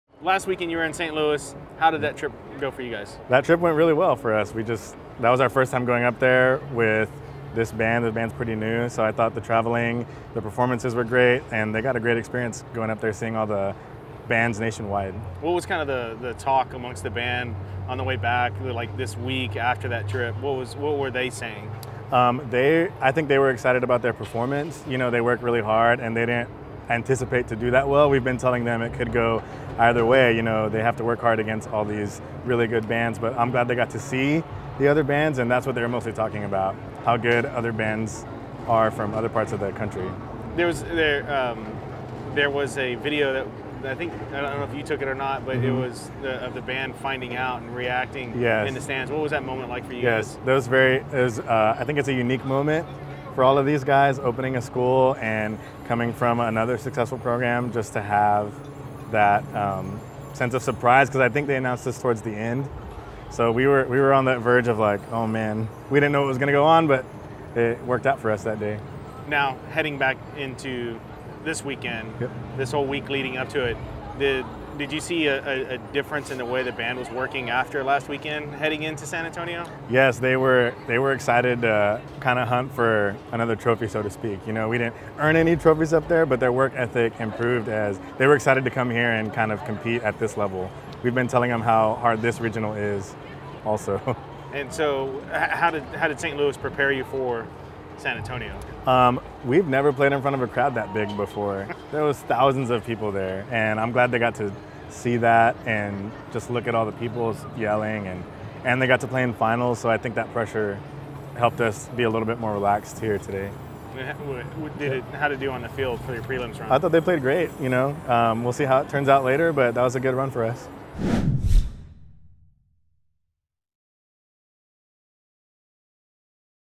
2018 Weiss Marching Band: Director Interview w/ Jordan Garza
Exclusive interview with Weiss High School Band Director, Jordan Garza, during the 2018 BOA San Antonio Super Regional.